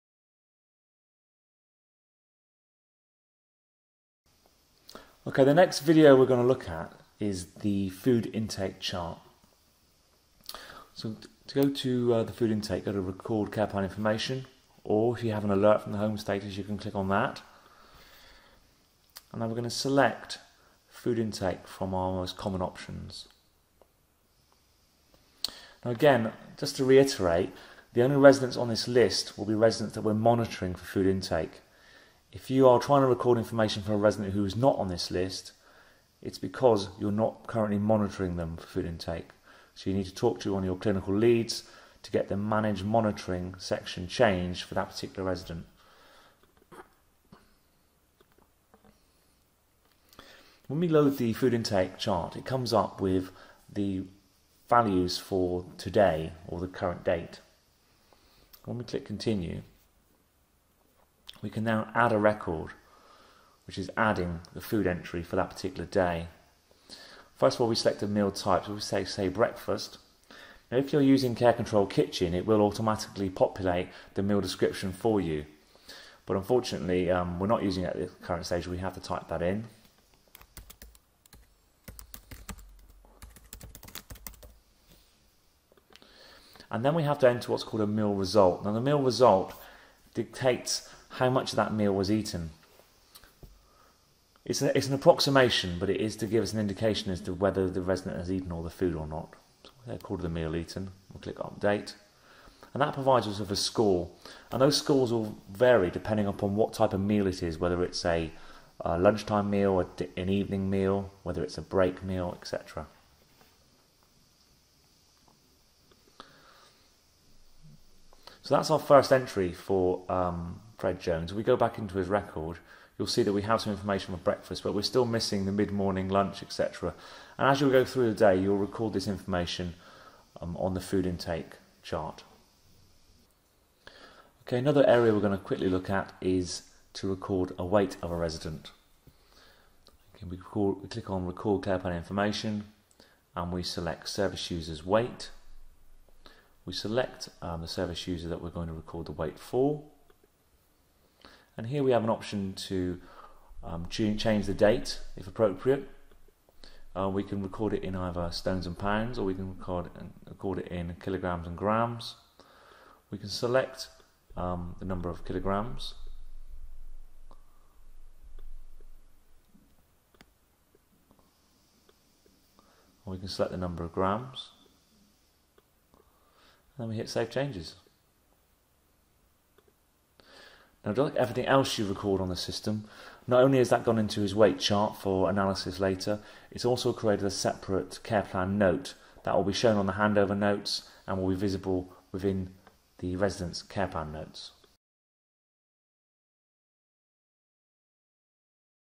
Food Intake, Weight Recording
Explains how you can record food intake and weight records.